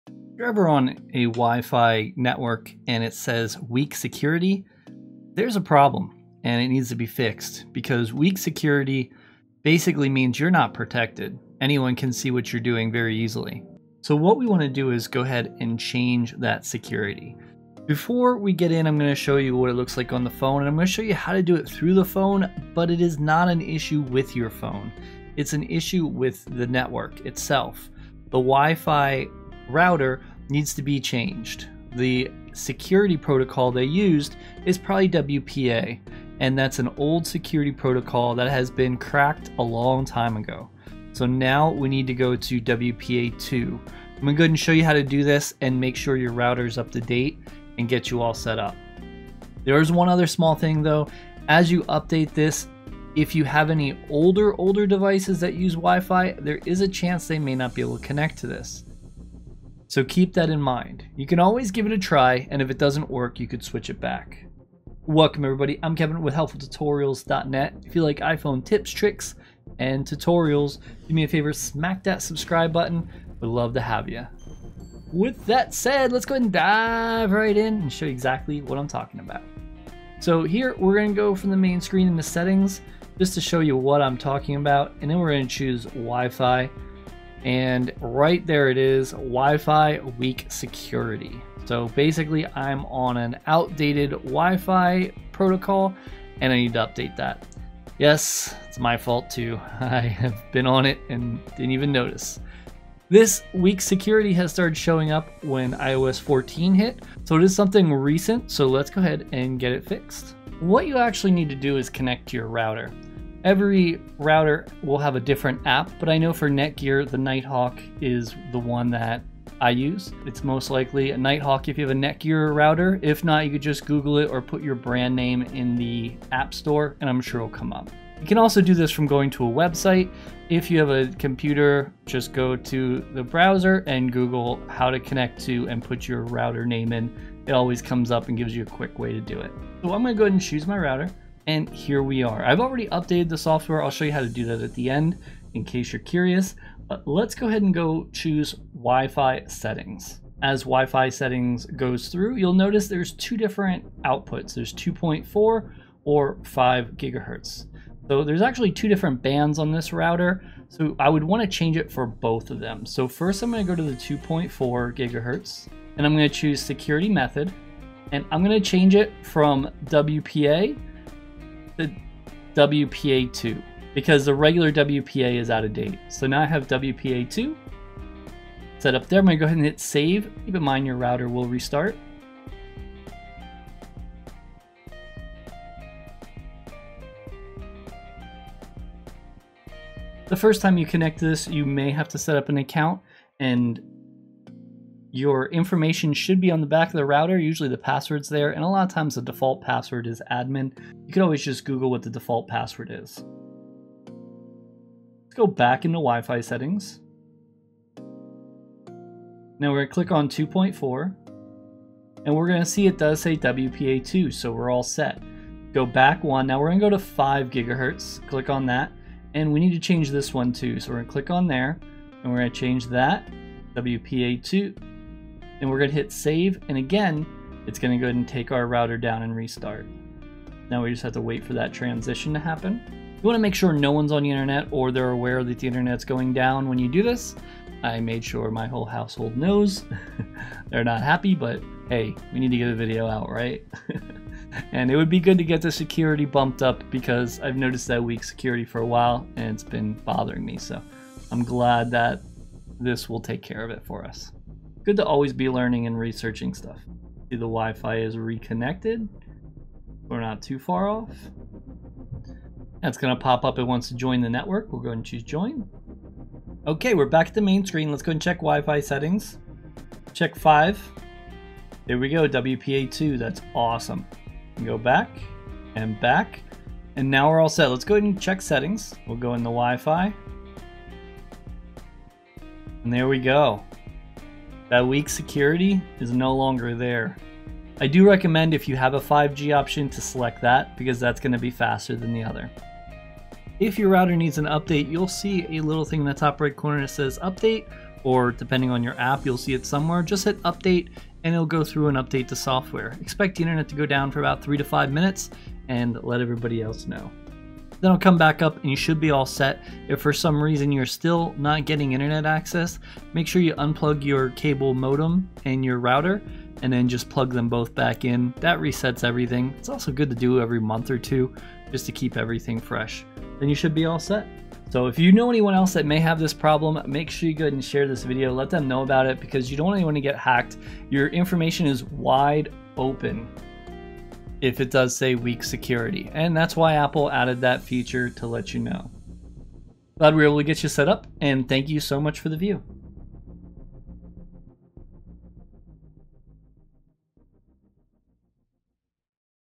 How To Fix Weak Security On Wifi & Why it's URGENT!
How To Fix Weak Security On Wifi & Why it's URGENT! If you are seeing "Weak Security" when connecting to WiFi you should be careful. If it is your home WiFi I'll show you how to fix it so other people can't steal your data. This started to happen on iOS 14 because Apple made everyone aware, it's basically because WPA is out of date and has been cracked, so you need to update to WPA2.  In this video, I'll show you how to do this but also VPNs can also help keep you secure: